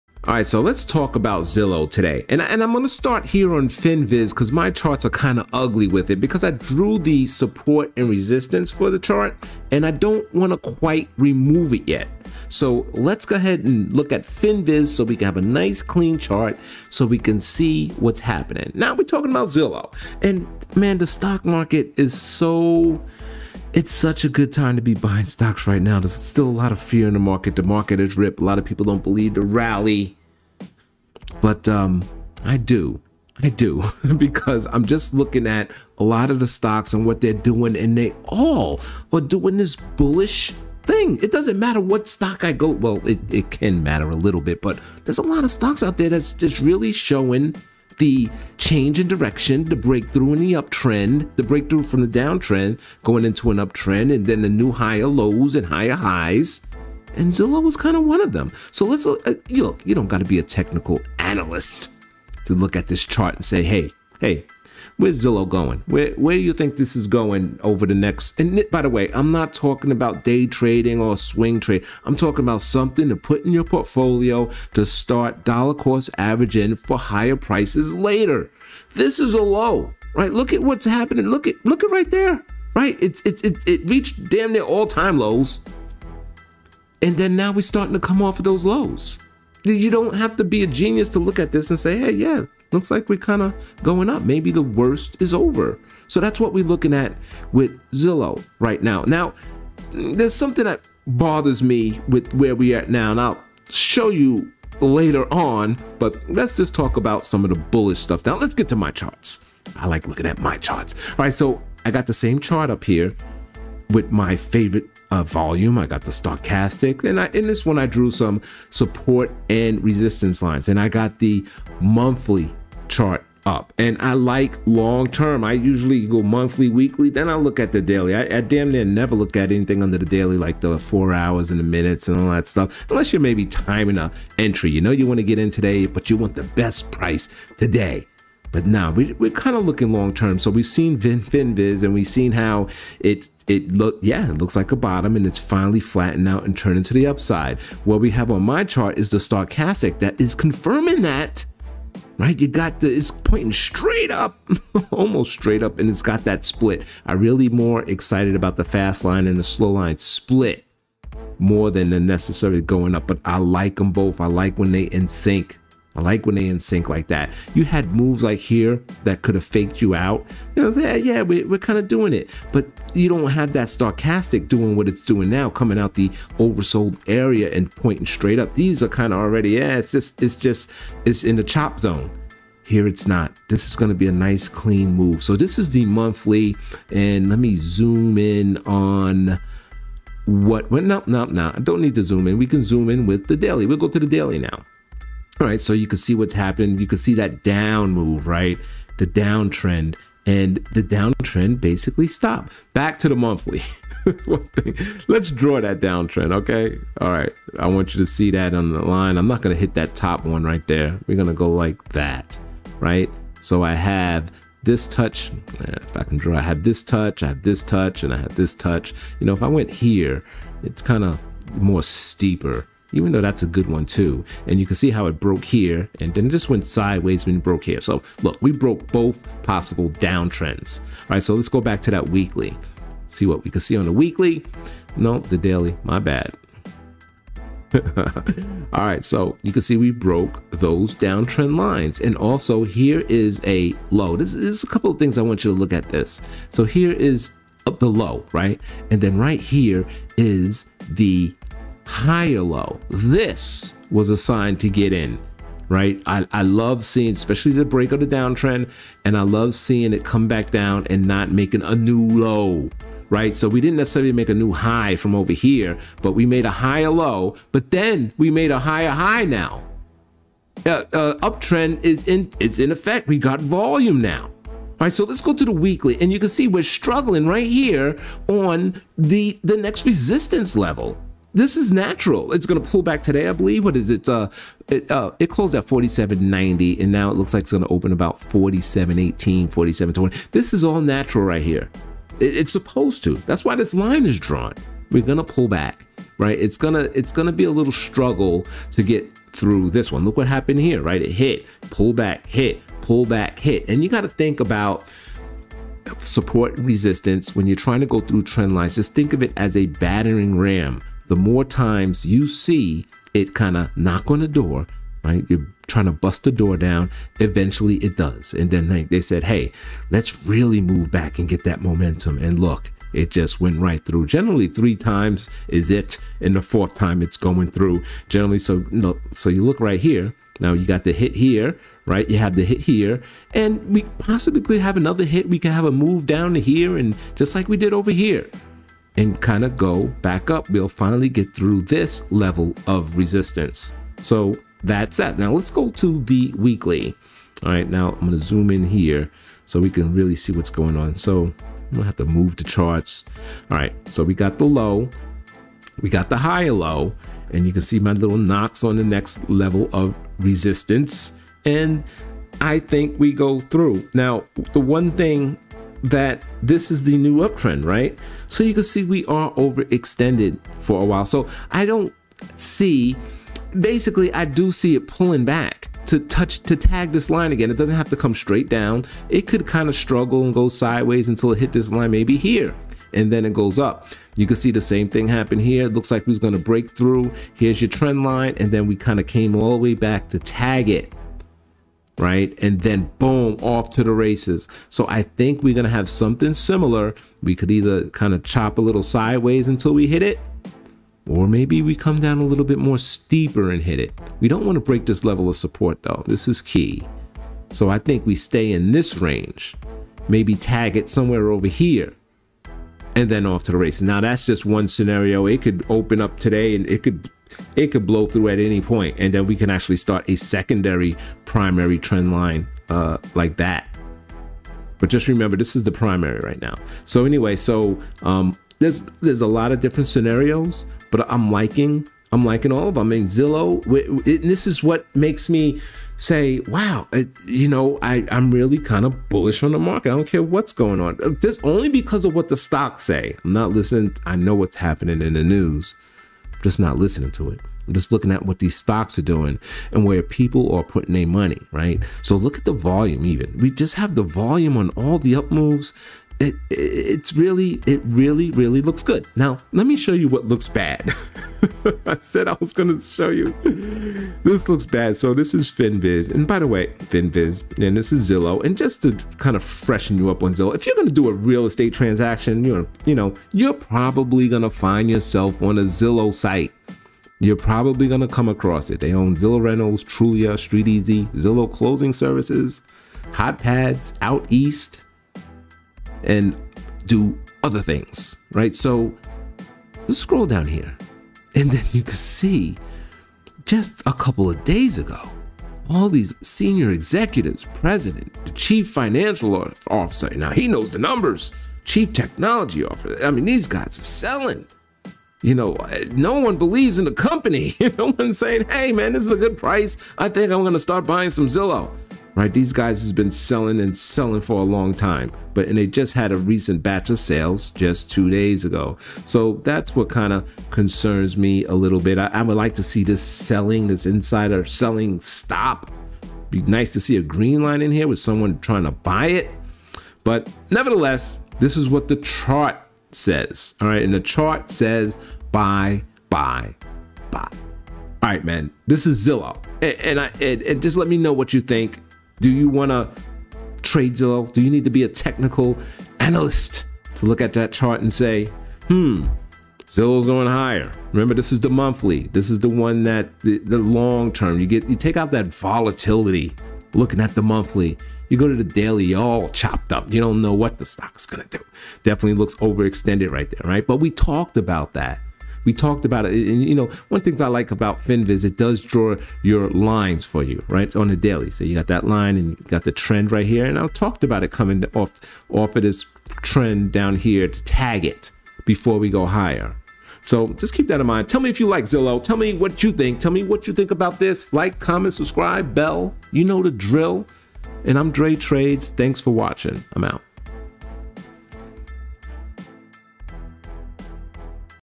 Zillow Stock Price Prediction along with Zillow Technical Analysis
Discussing Z stock showing you that the down trend in the stock could be over.  Yes, we may still go down but the risk is to the upside.